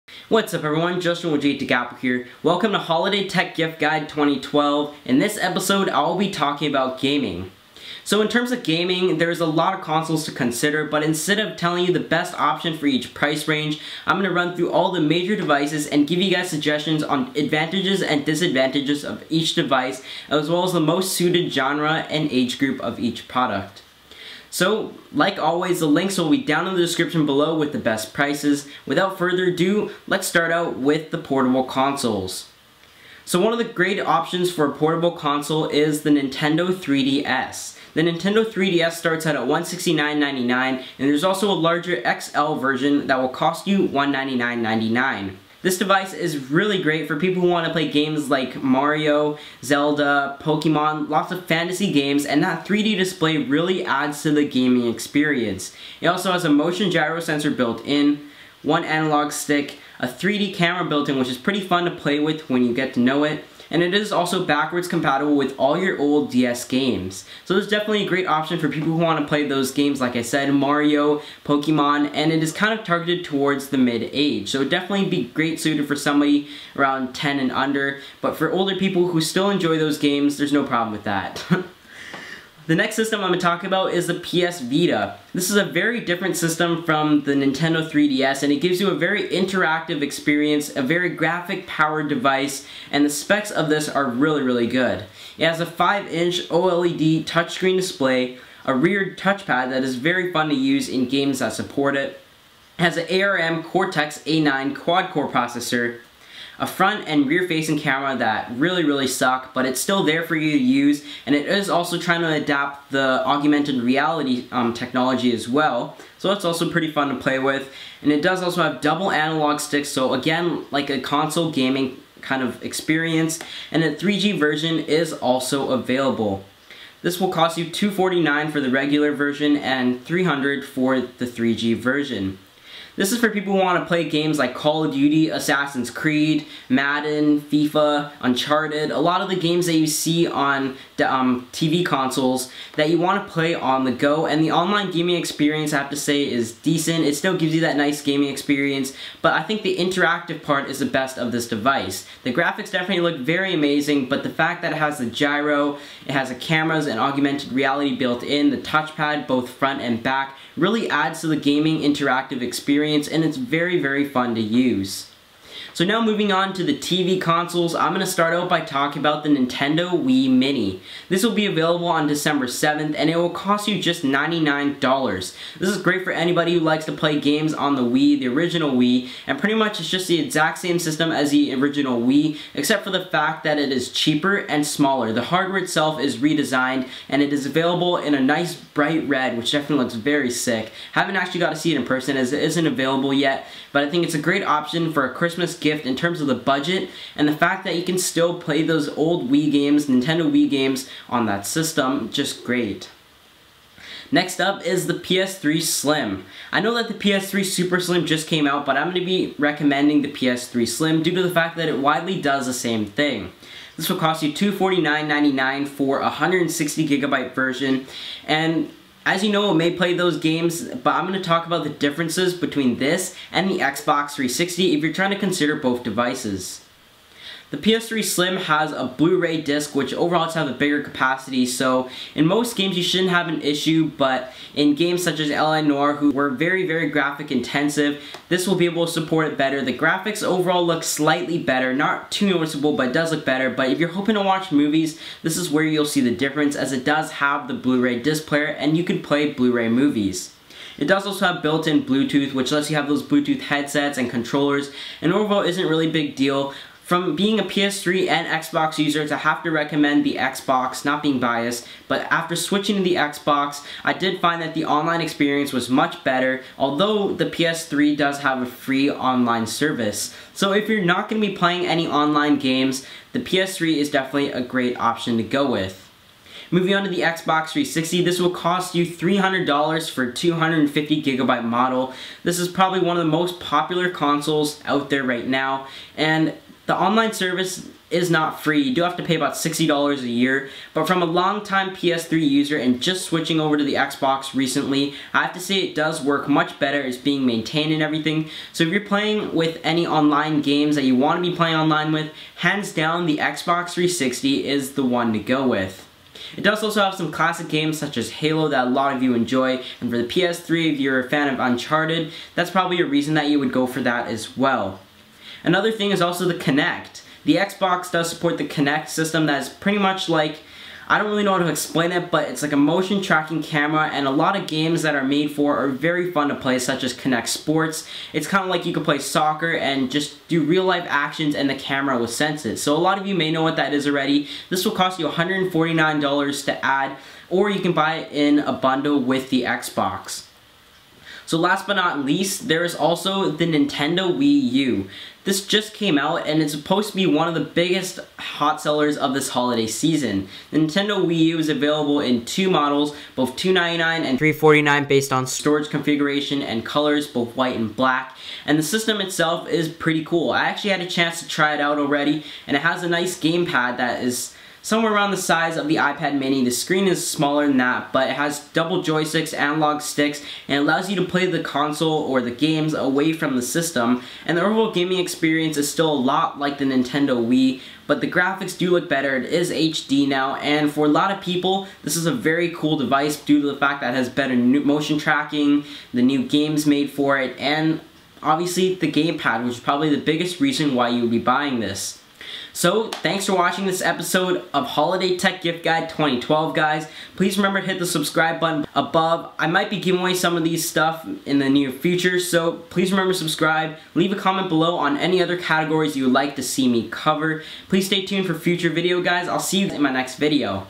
Holiday Gift Guide 2012: Gaming
BEST PRICES LISTED BELOW:

Nintendo 3DS

Sony PS Vita

Sony PS3 Slim

Microsoft Xbox 360